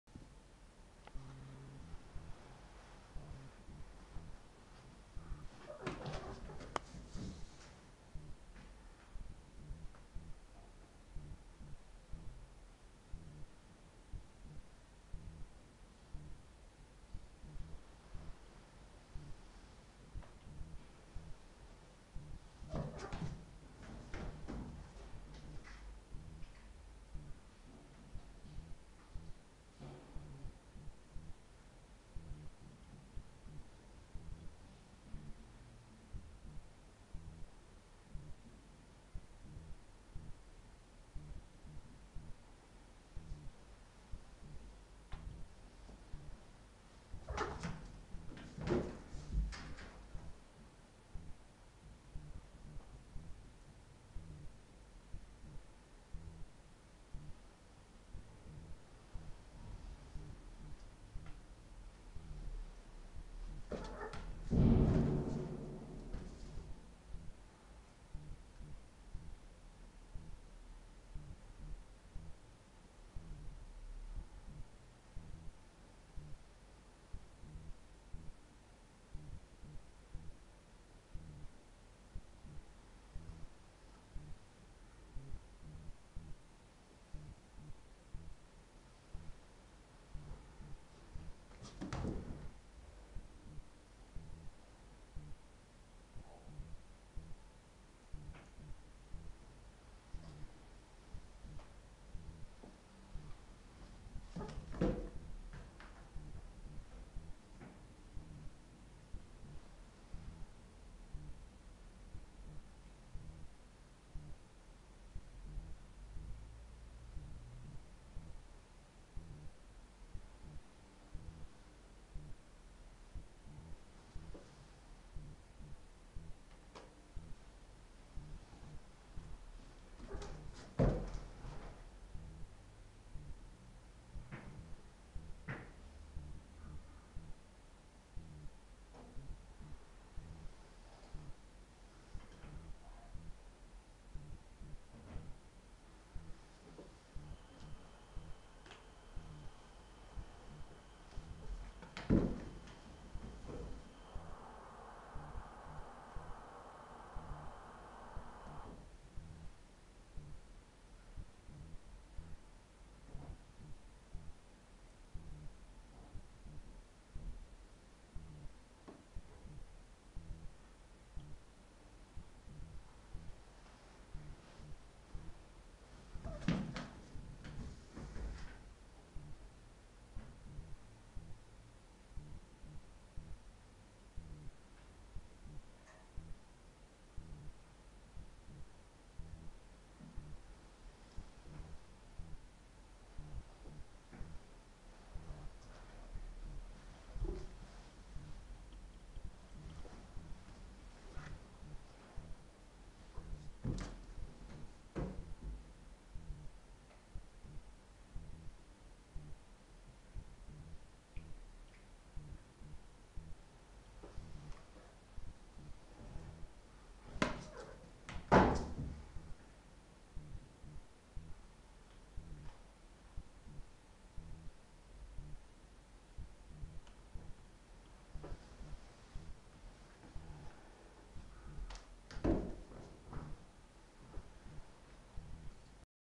Dunkin Donuts and Brooklyn playing ball
Kitten fetch time, Now Dunkin Donuts is just showing off!